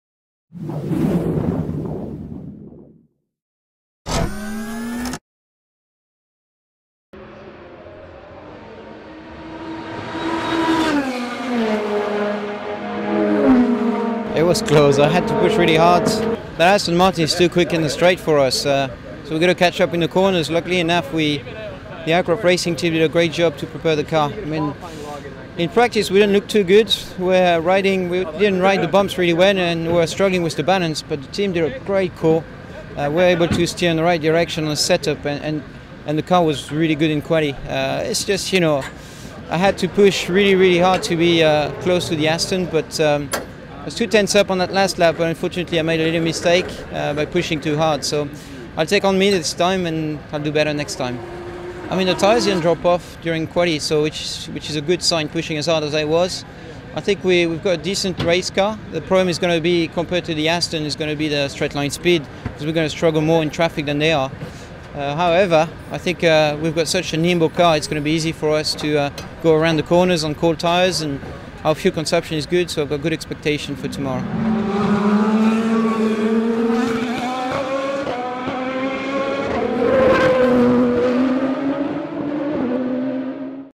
Long Beach Qualifying
Simon Pagenaud just missed out on claiming pole position on the streets of Long Beach -  beaten by only .072 of a second.